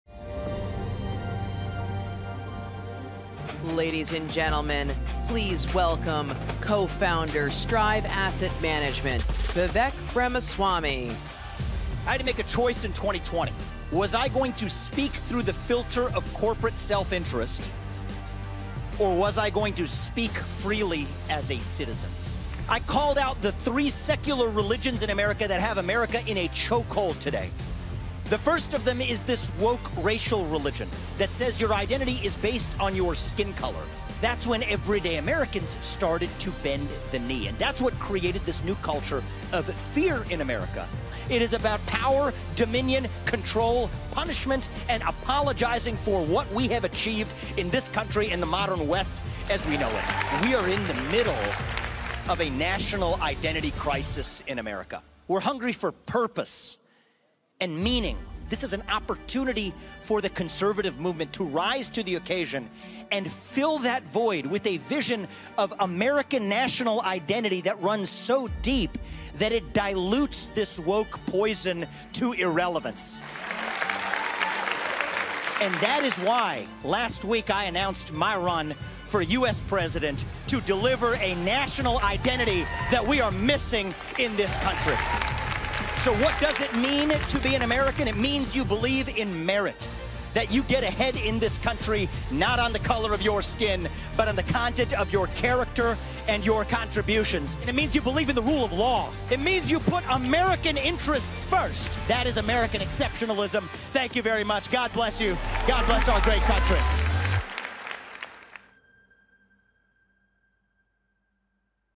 CPAC 2023: The Great Uprising
Time for The Great Uprising to defeat The Great Reset. This isn’t just an R vs. D question in 2023. It’s a 1776 moment.